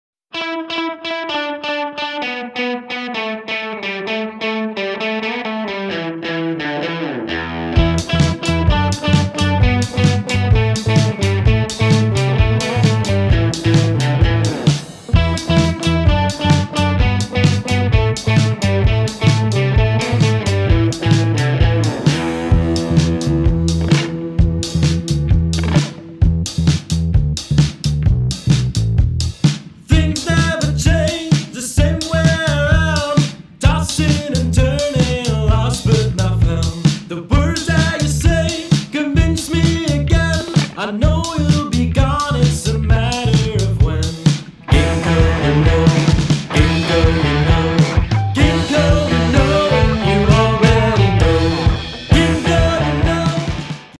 Teaser finale T strijd
Rocket Balloons will be performing at the final round of the T-strijd (band contest) on 26/03/2016 in Gebouw T, Bergen-op-Zoom, NL